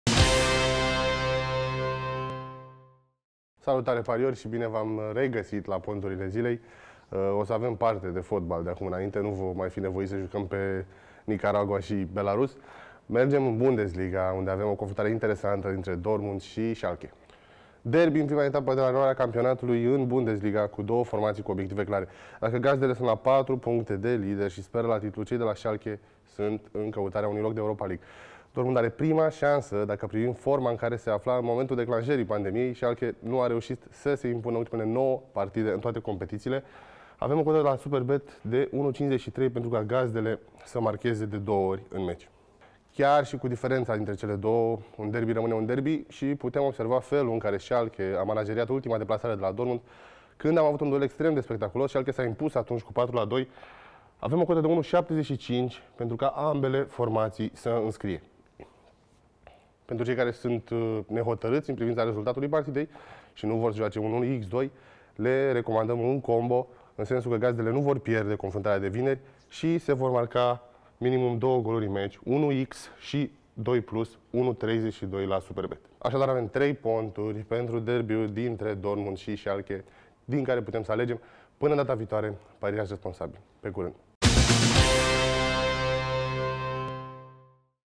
3 cote pentru cel mai important duel din Bundesliga » Faci bani la Dortmund - Schalke
Bundesliga revine în forță - Borussia Dortmund - Schalke 04 este derby-ul etapei, sâmbătă, de la ora 16.30. Va fi cel mai ciudat derby din istorie, a declarat fostul jucător și actualul director sportiv al Borussiei Dortmund, Sebastian Kehl.